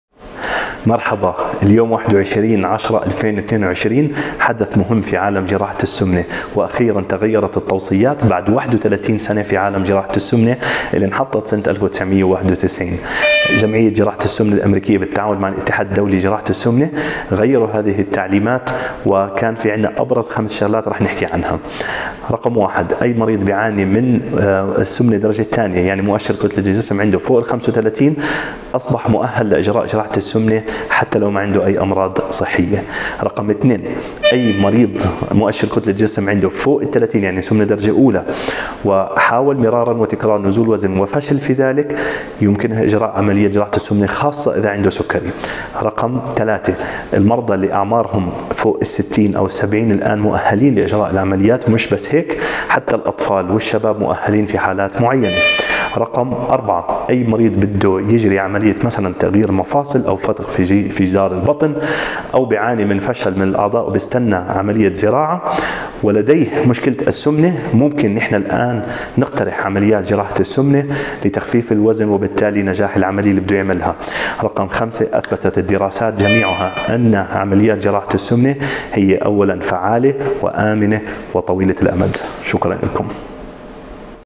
التوصيات العالمية الجديدة في عالم جراحة السمنة | الدكتور حمزة الحلواني
نشر التوصيات الجديدة من الجمعية الامريكية لجراحة السمنة بالتعاون مع الاتحاد الدولي لجراحة السمنة وذلك بعد 31 عاما من التوصيات القديمة. التغييرات شيقه ومبنيه على ثلاث عقود من الدراسات المستمرة تابع معنا ما الجديد في هذا الفيديو.